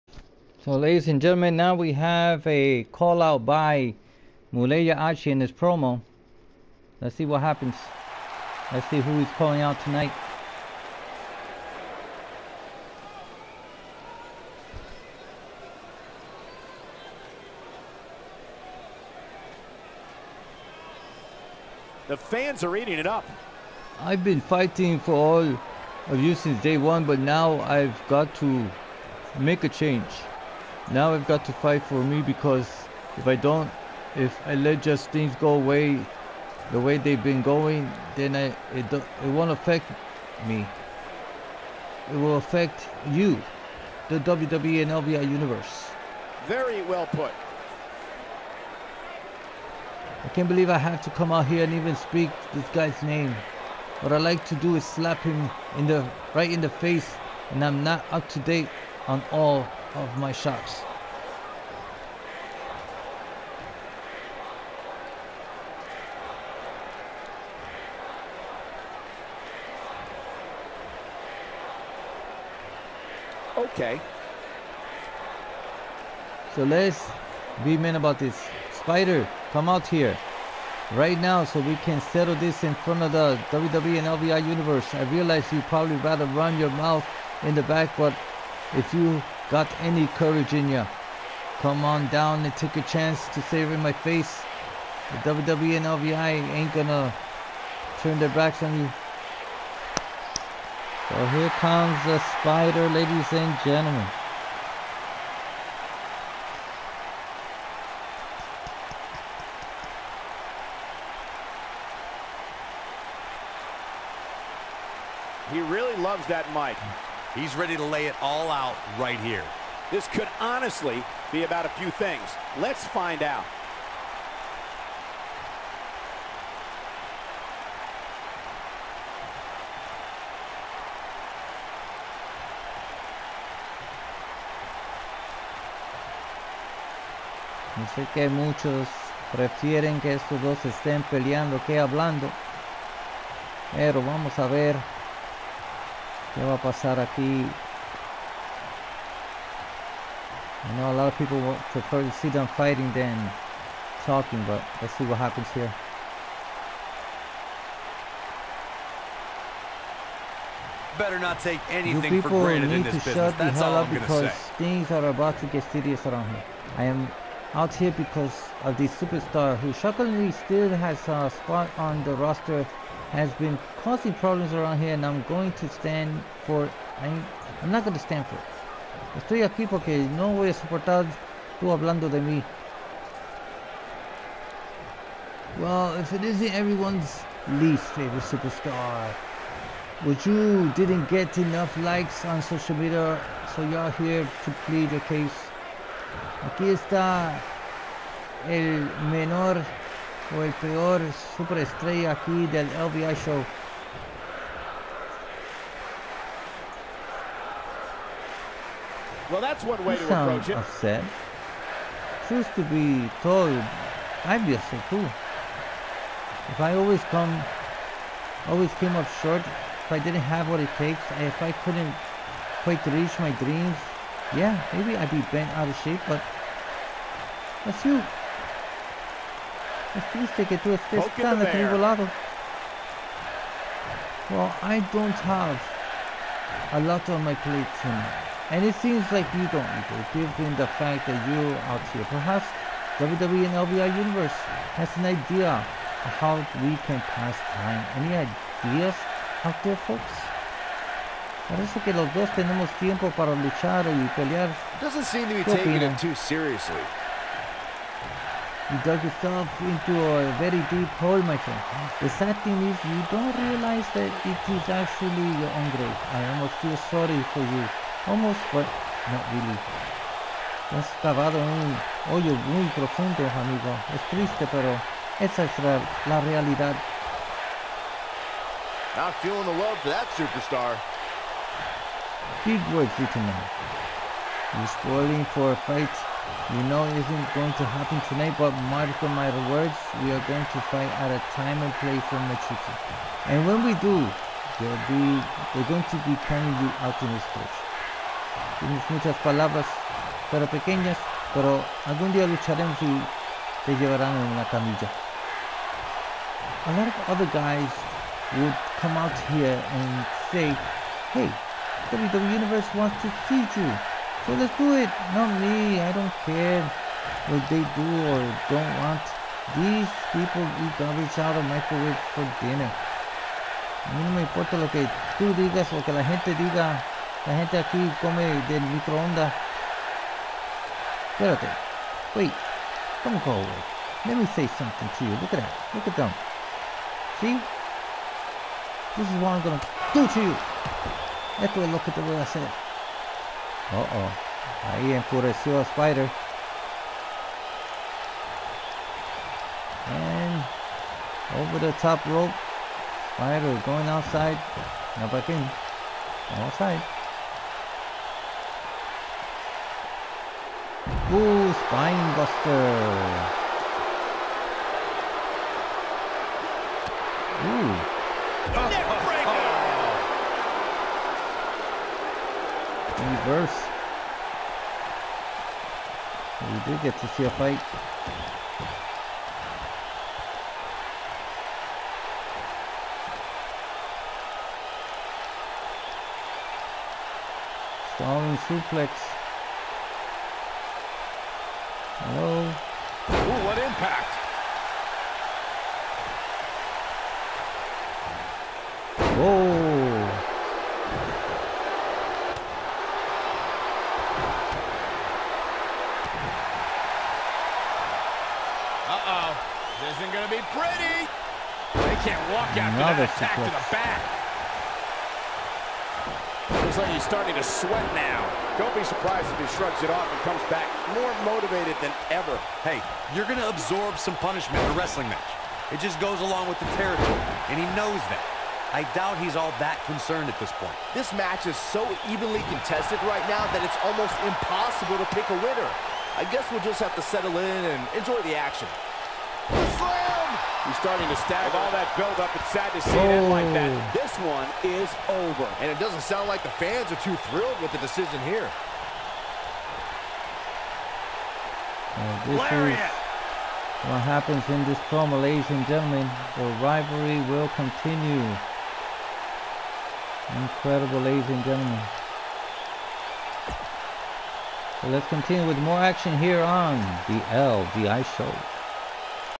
MULEYA HACHY VS THE SPIDER!!
The following is Muleya Hachy's promo, where he calls out The Spider and they ended up in a brawl.